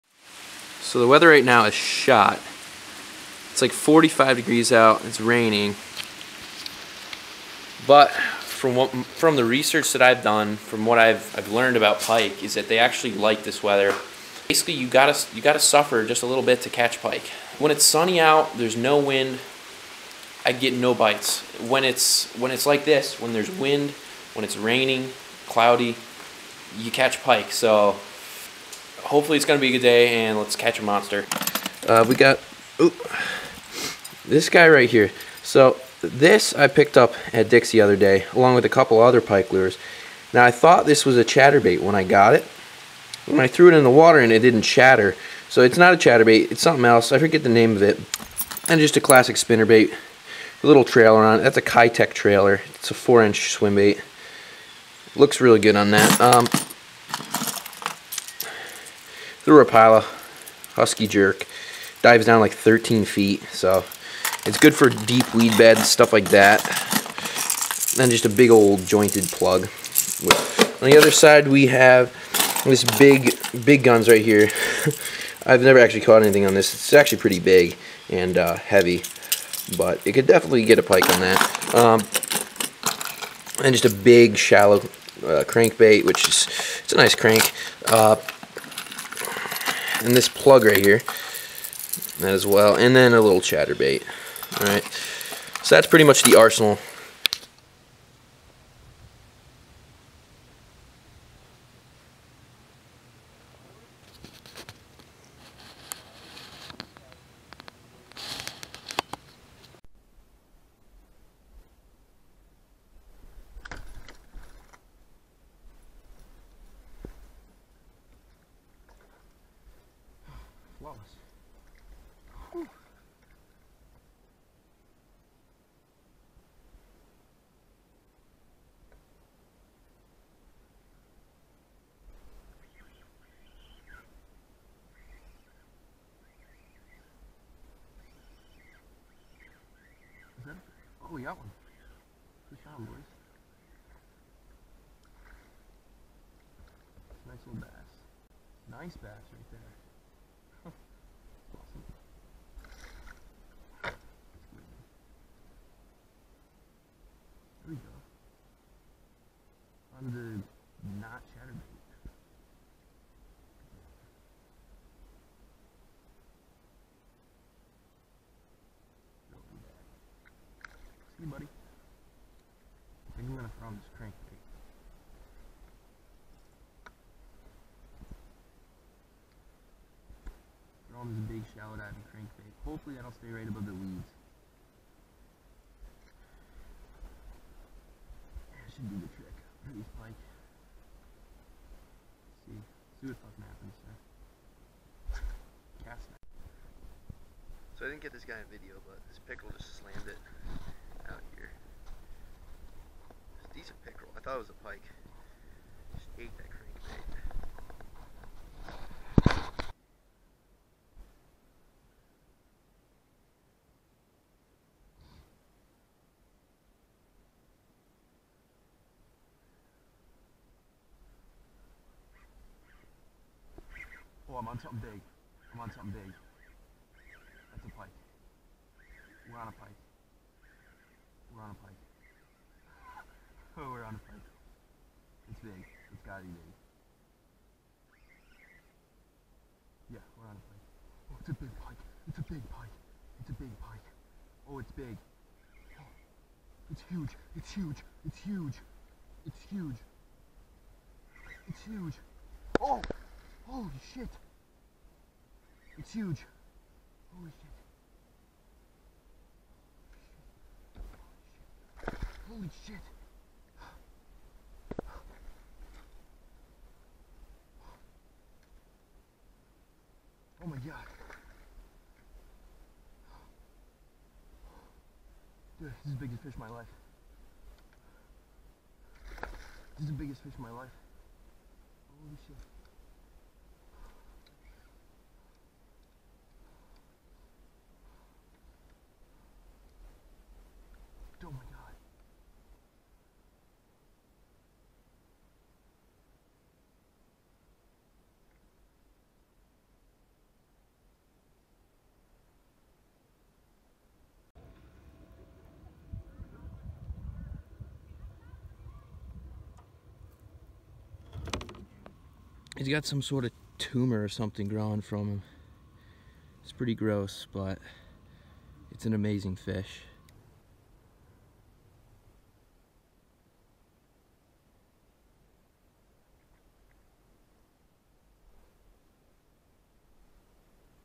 Catching My Largest Northern Pike in a Small Lake - Upstate NY
I Caught my largest northern pike in a small local lake. Unfortunately it had a huge growth coming out of its side near the gills. I'd estimate the fish to be around 35 in. It was caught with a big, shallow diving, square-bill crankbait in 10 ft of water. More videos of pike, bass, trout, and panfish fishing coming soon. Ice fishing videos will also be coming this winter.
Reel: Diawa BG 3000
Rod Ugly Stick GX2